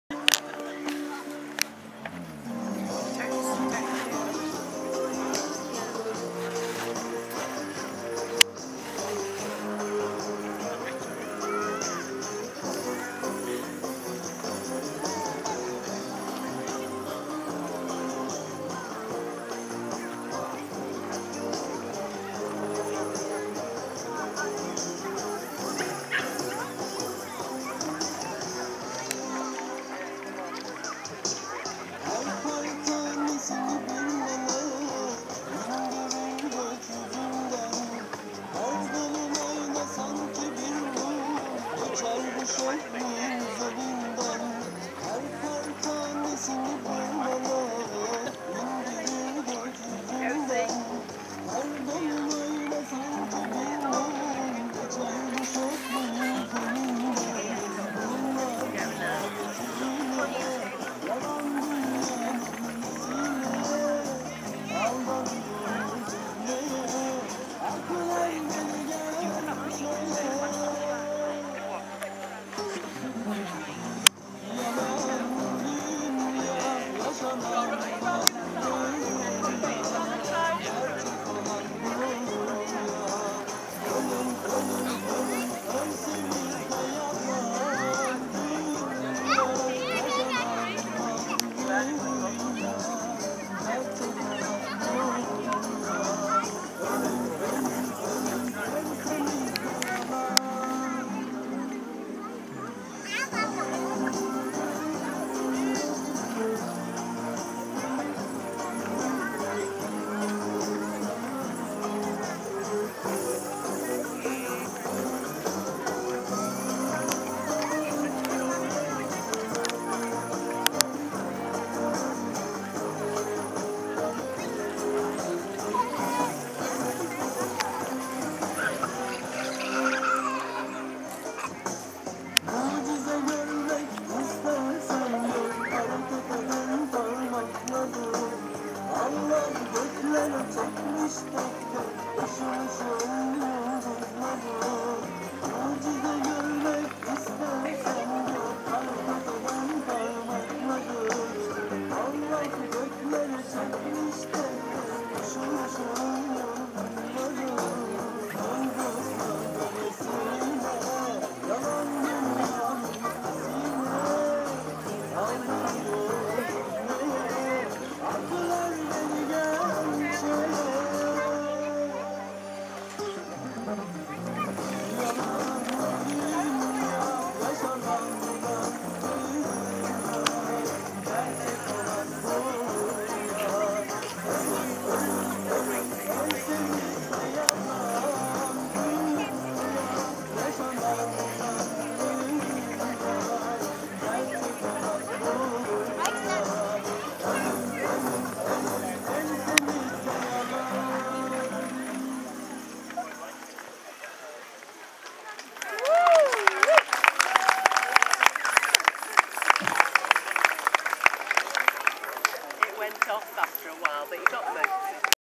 Belly dance ashton park 2014 group dance
May day bank holiday 2014.  Ashton Park West Kirby Wirral UK.  Group middle eastern dance